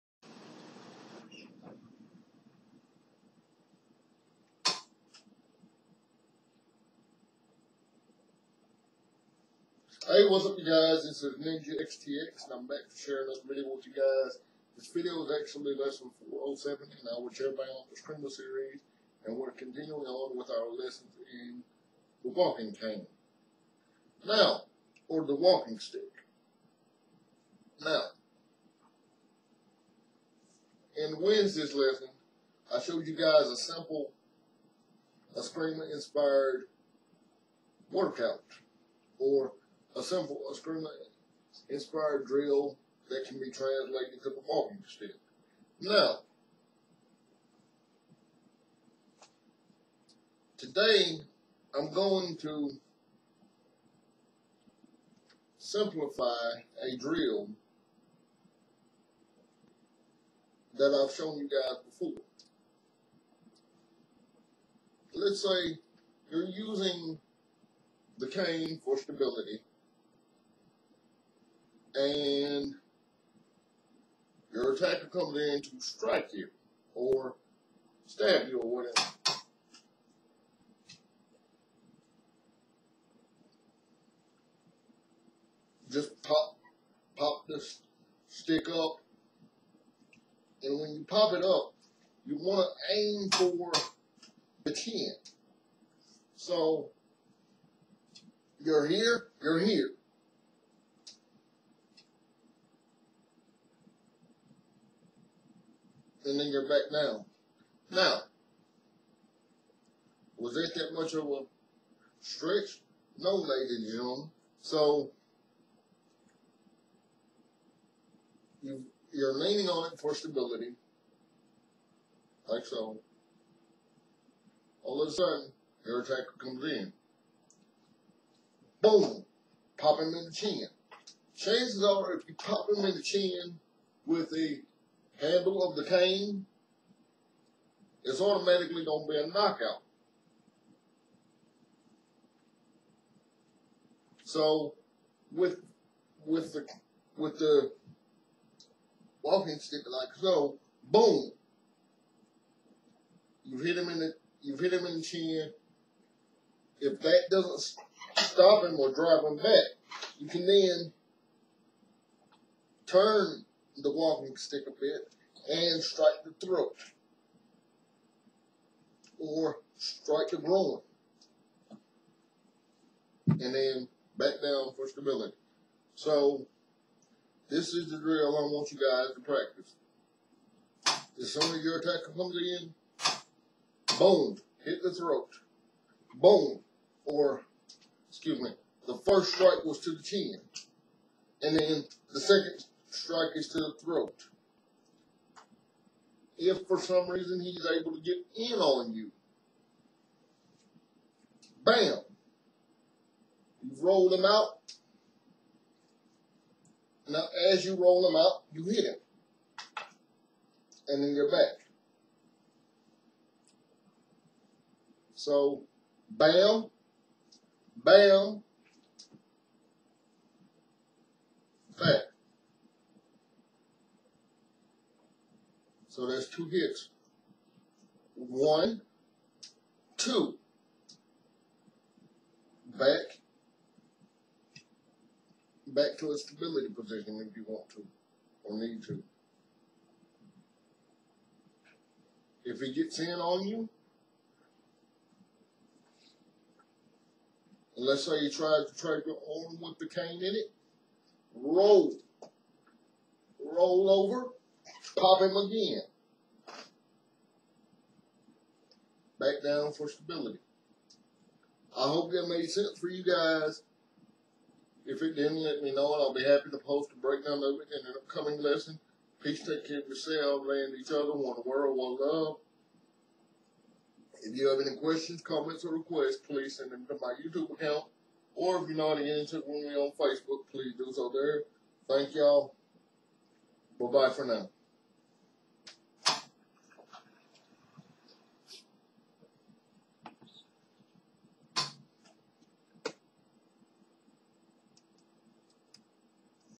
Chairbound Escrima Lesson #407 4-24-2015.
Chairbound Escrima Lesson #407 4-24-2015. . This Morning's lesson focuses on a Simplified Walking Stick Drill. Please Enjoy, Give Feedback And Remember To Practice Practice Practice